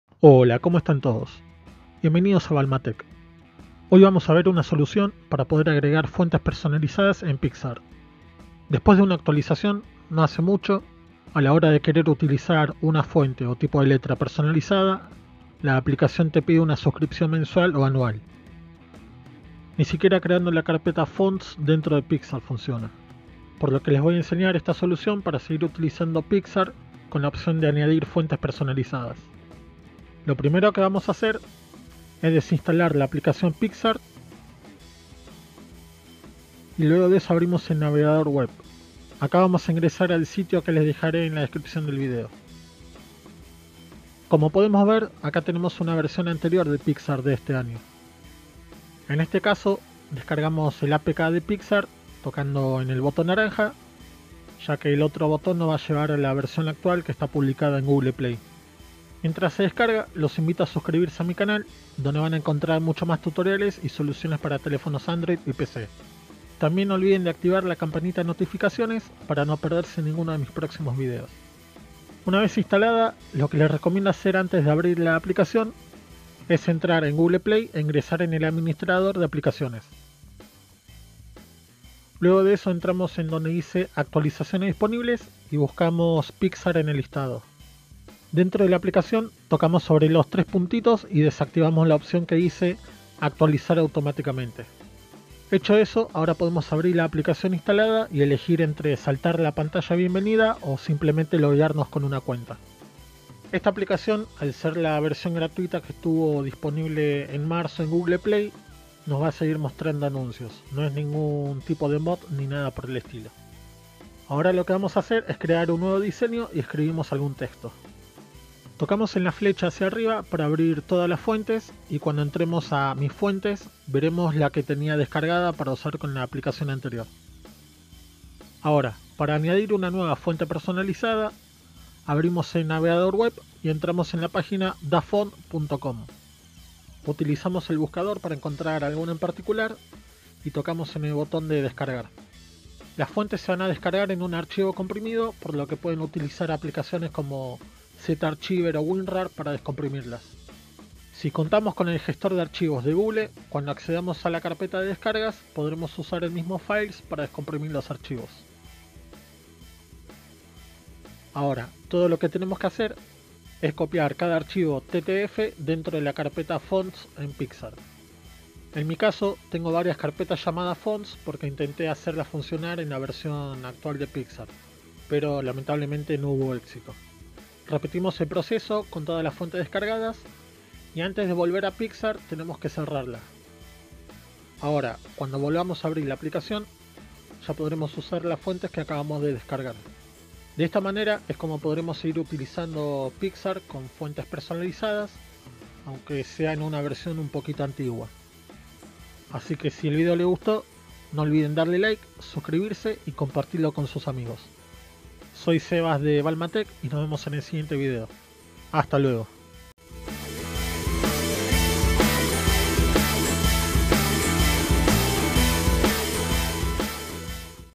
Cómo descargar e instalar fuentes personalizadas en PicsArt para mejorar tus diseños en Android
Aprende cómo descargar e instalar fuentes personalizadas en PicsArt para mejorar tus diseños y darles un toque único y original. En este tutorial, te mostraremos cómo obtener fuentes gratuitas y cómo agregarlas a tus proyectos en PicsArt en pocos pasos. Descubre cómo personalizar tus diseños y sorprender a tus seguidores con tipografías creativas y originales. ¡No te pierdas esta guía completa sobre cómo descargar e instalar fuentes en PicsArt!

☑️ Tutorial para descargar e instalar nuevas fuentes en PicsArt

☑️ Ya sabes escribir con diferentes tipos de letras en WhatsApp???